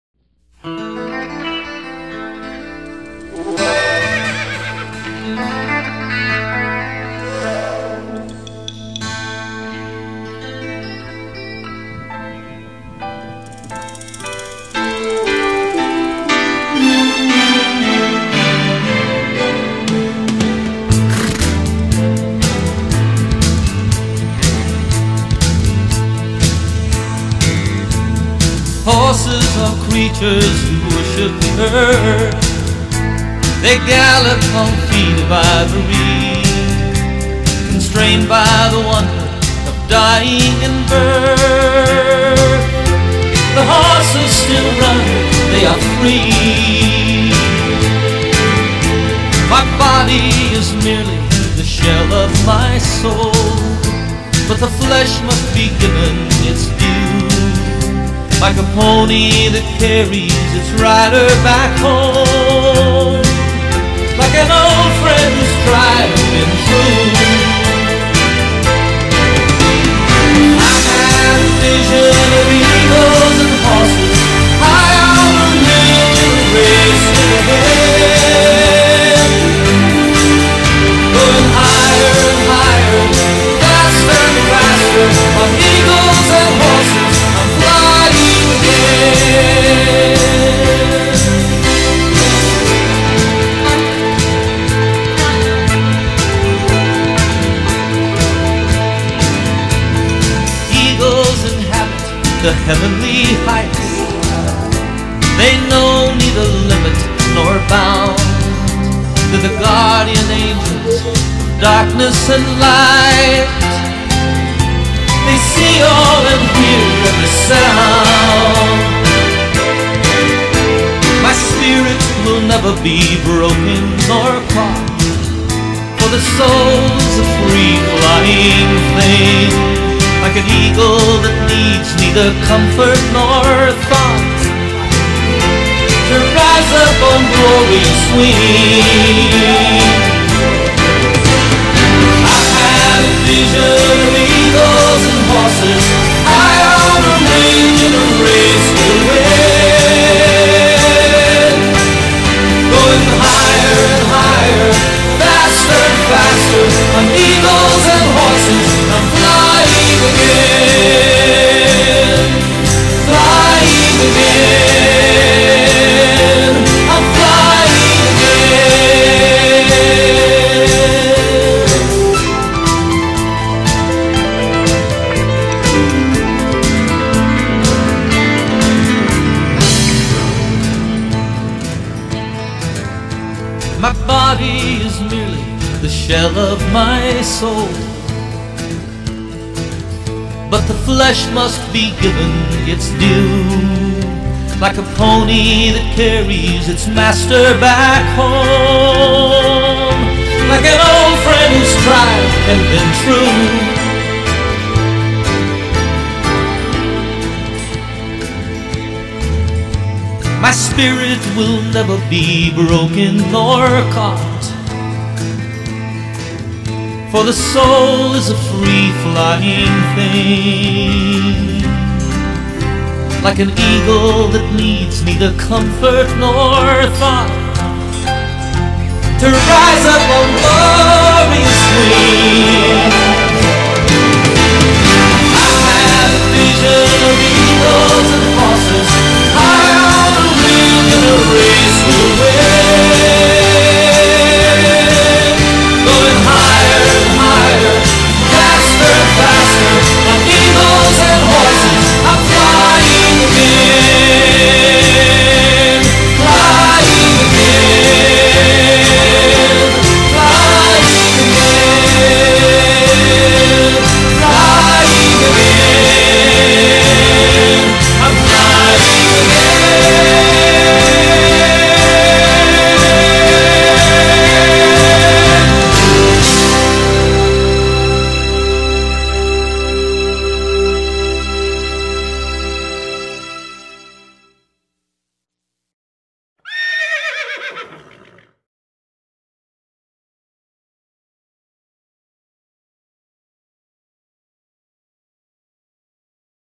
Eagles and Horses
After 35 years of praying, Dad and I have horses again.  Merry Christmas to all  who understand the feeling I have tried to capture in this video.  My Dad was 72 years old before he ever saw a bald eagle.  So many of life's blessings have been shared with him.